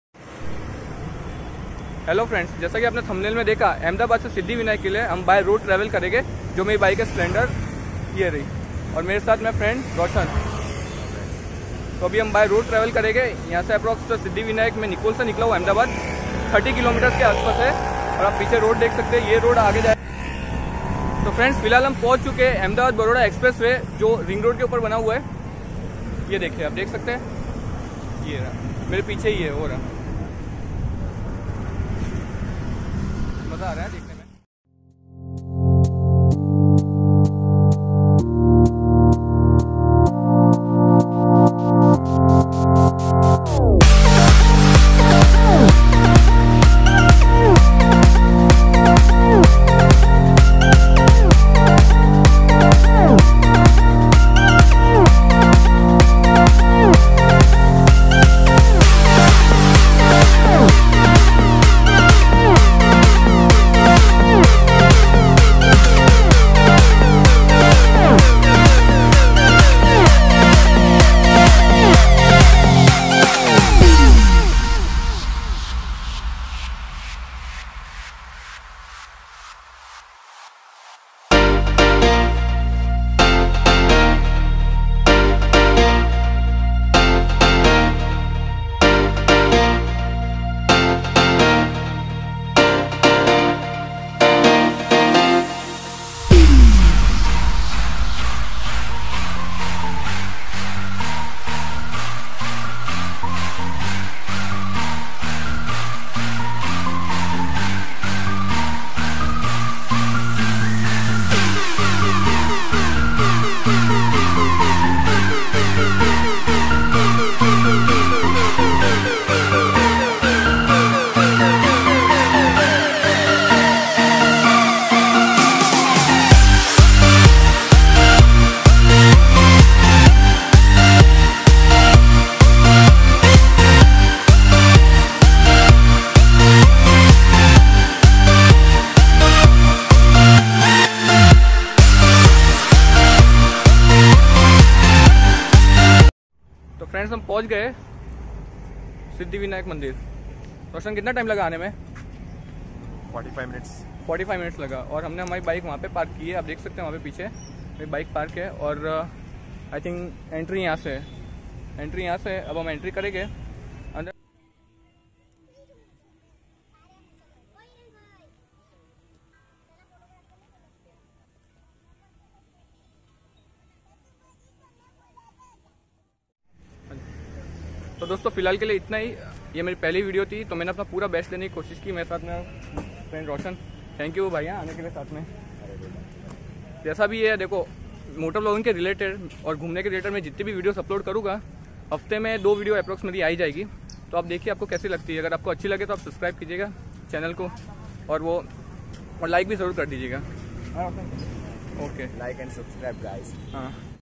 Ahmedabad To Siddhivinayak Mandir Mahemdabad By Road (Splendor 100CC bike) it's my First Video
Hello Friends My is Name Gaurav and I share my riding experience. And it's my First YouTube video

Gorilla tripod

Support me

Category:- Travel & Event